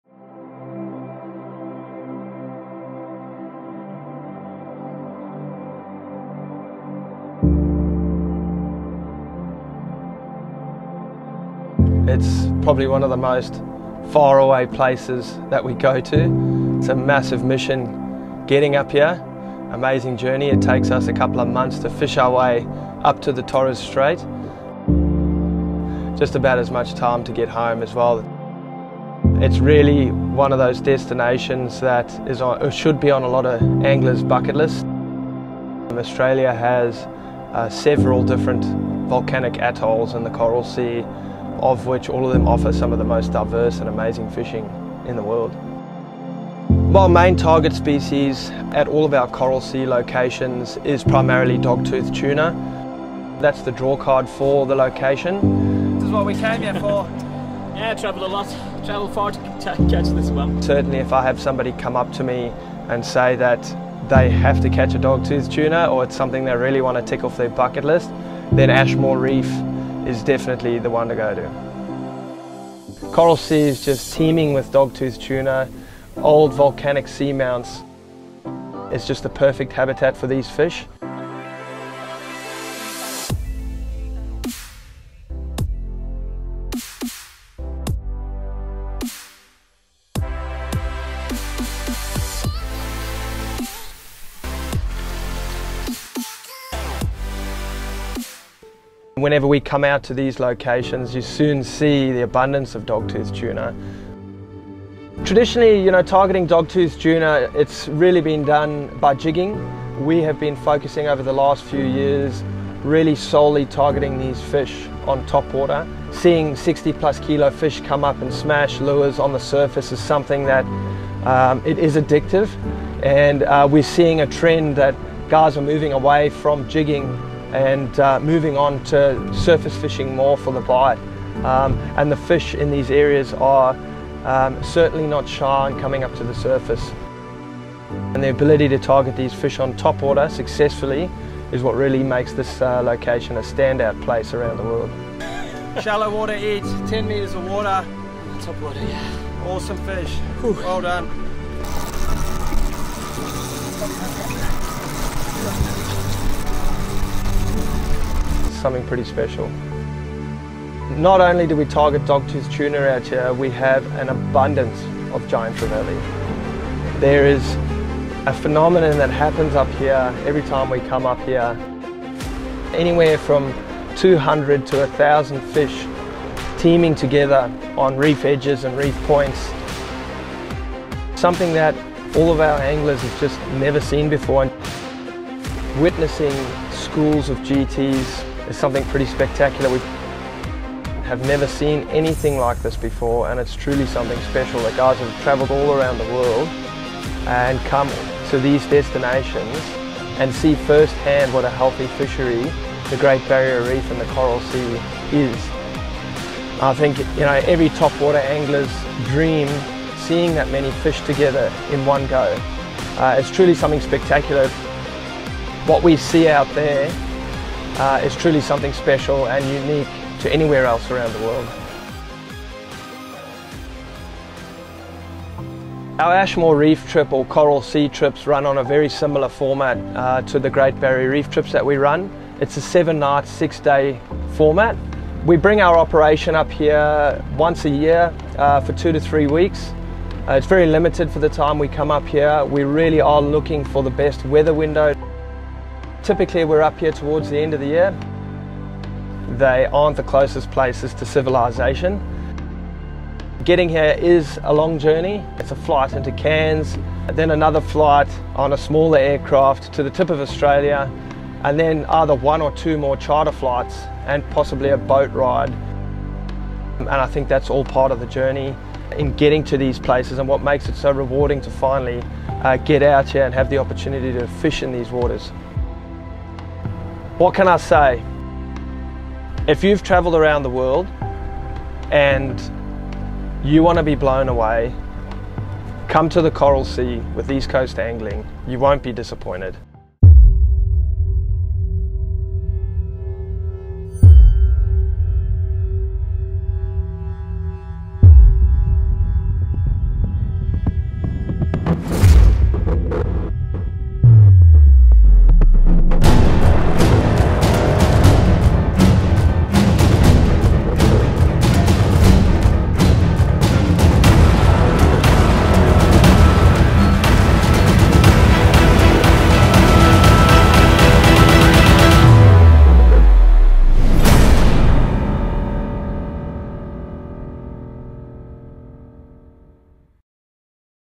Giant Topwater Dogtooth Tuna, Ashmore Reef
Ashmore reef is home to giant Dogtooth Tuna. We specialise in chasing doggies on top water on our Coral Sea, Torres Strait mothership trips.
It is not just Dogtooth tuna at Ashmore. There are schools of hundreds to thousands of Giant Trevally that create havoc on anglers' topwater fishing tackle.

Ashmore Reef in the Coral Sea is a fishing mecca like no other place in the world.